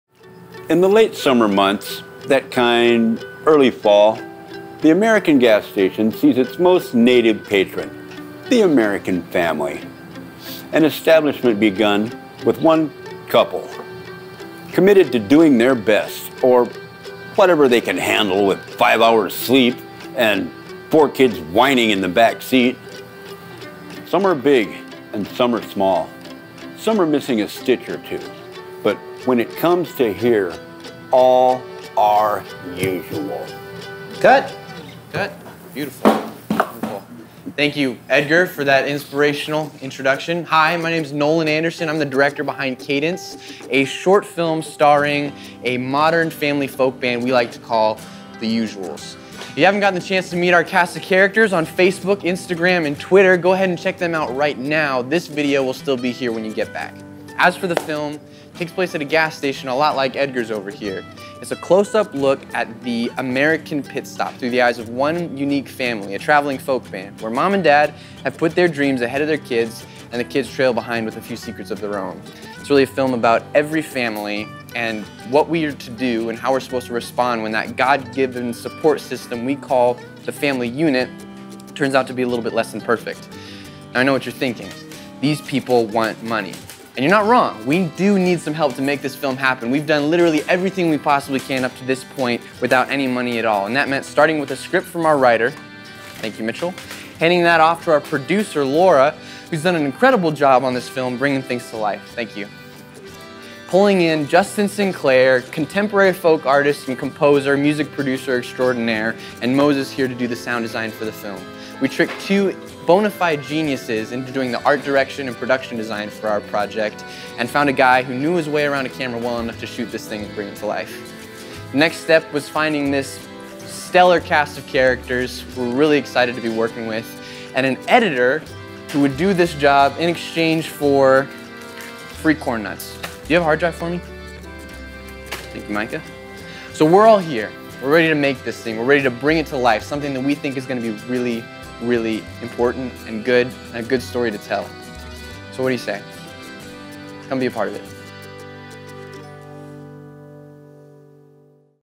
Cadence - Seed and Spark Video
Get to know the crew of Cadence! The story about a modern family folk band. Meet our director Nolan Anderson, as he talks to you about a cool project we are putting together. If you want to join us or help us make this movie... Go to our Seed and Spark page and donate.
We would love to hear from you! Enjoy!